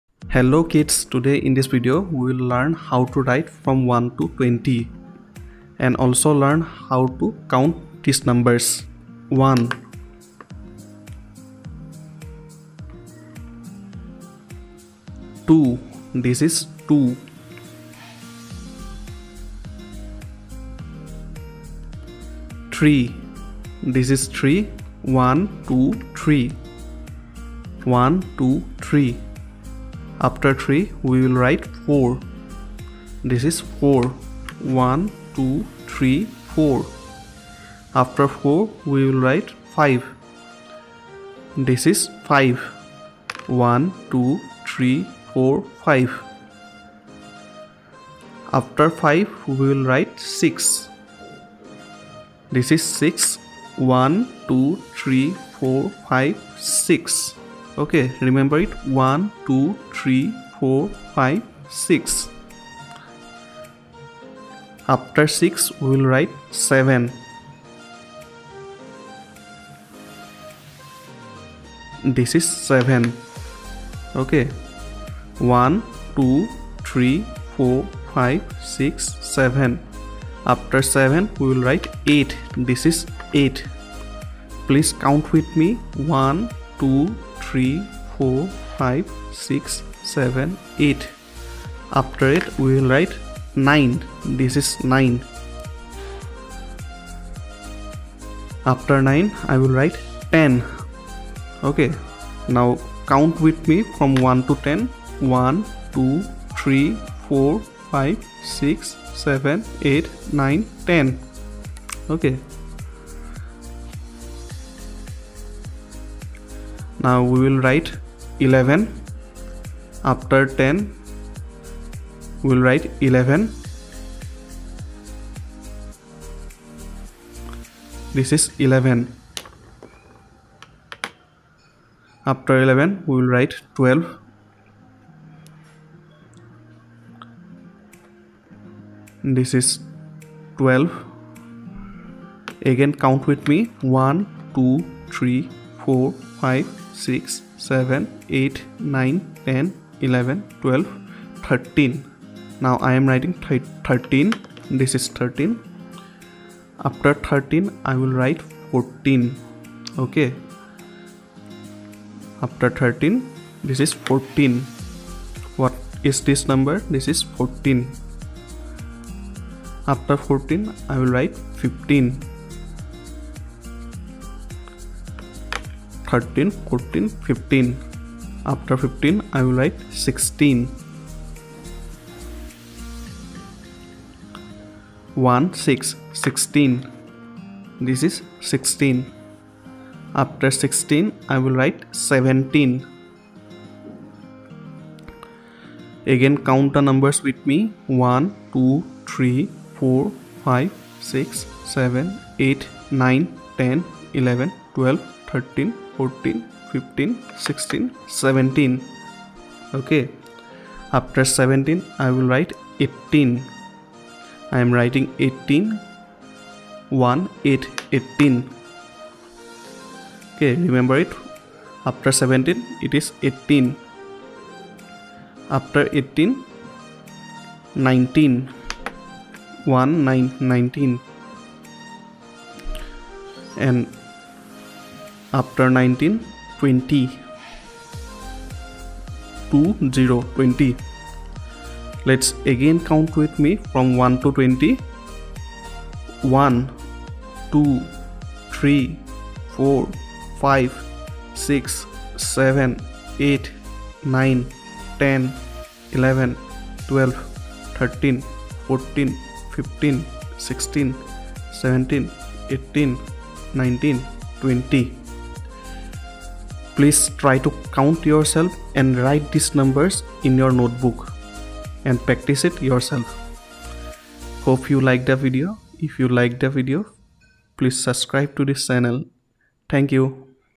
How to Write from 1 to 20 / Counting numbers 1 to 20 / Learning numbers from 1 to 20 / 12345
This video will help kids to learn how to write numbers from 1 to 20 along with counting the numbers from 1 to 20. I have teach numbers in a fun way so that kids can enjoy the video and takes knowledge of numbers.